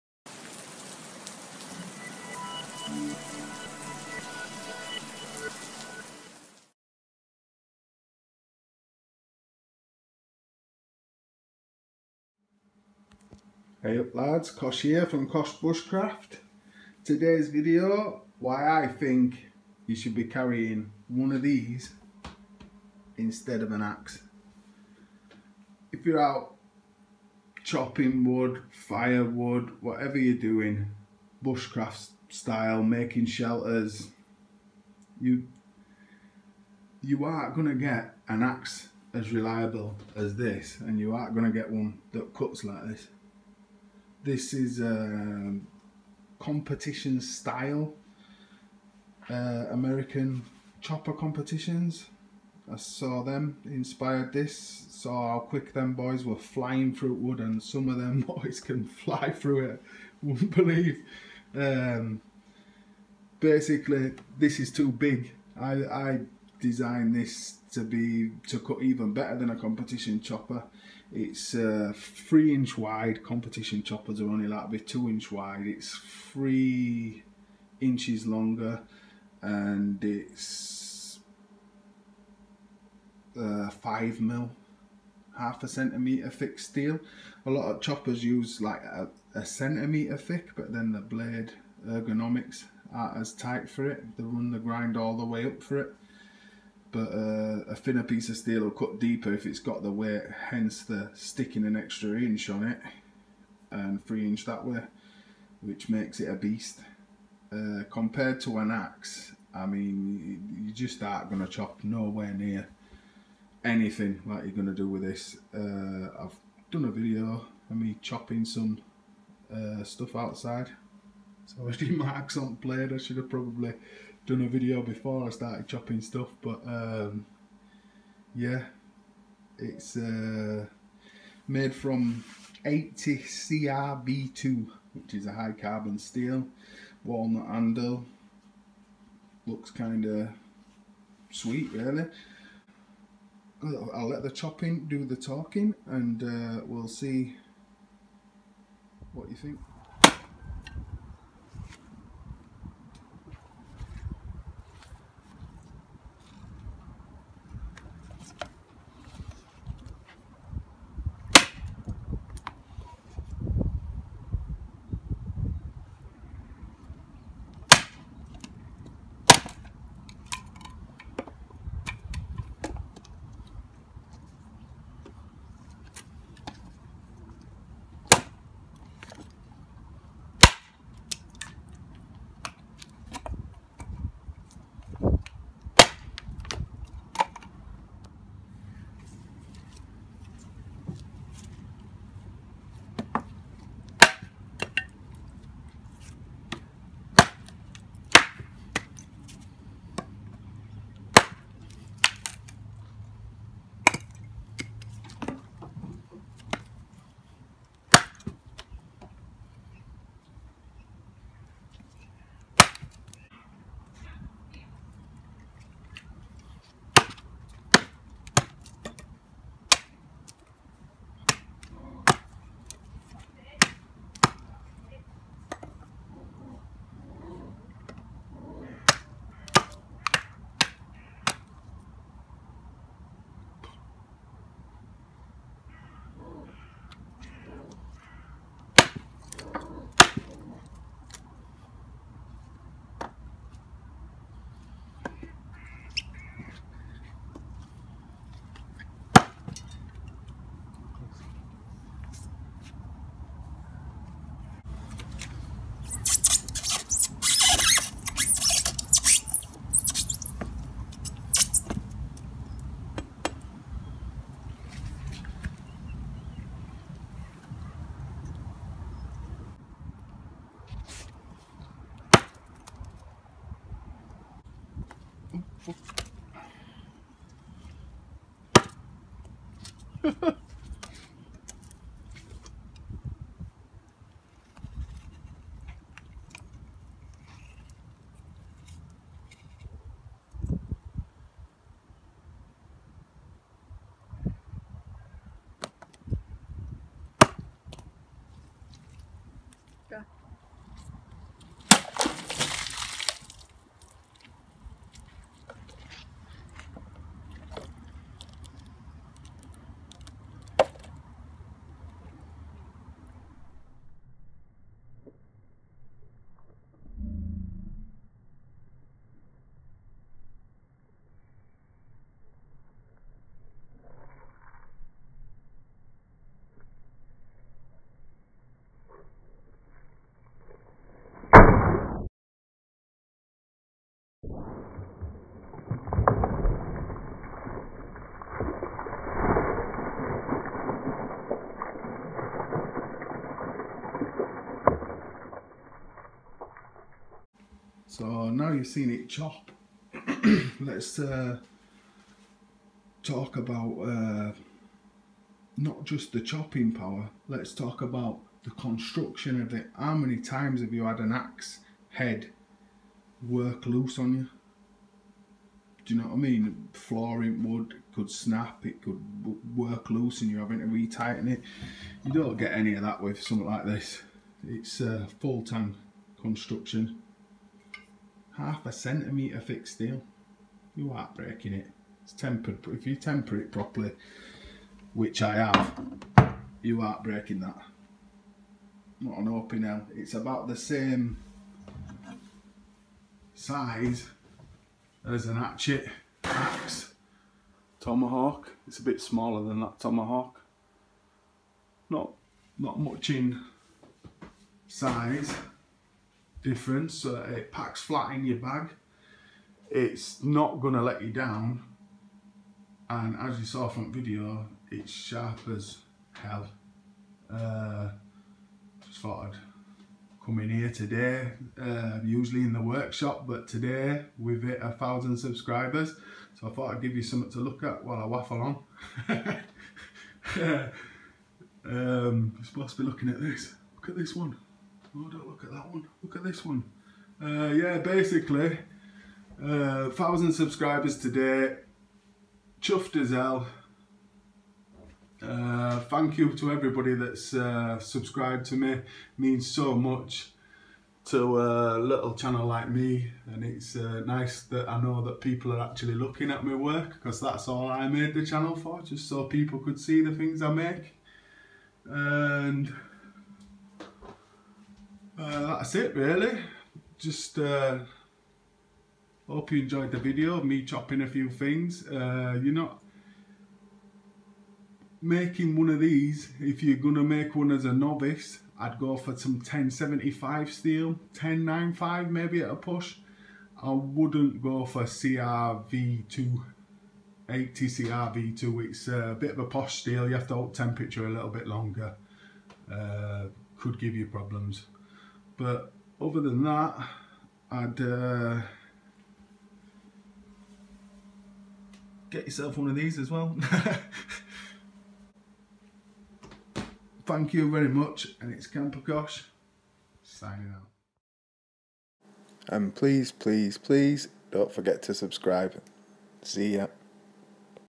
Modified axe why not make this instead!
Competition chopper style better than an axe?
Chops wood like nothing else way stronger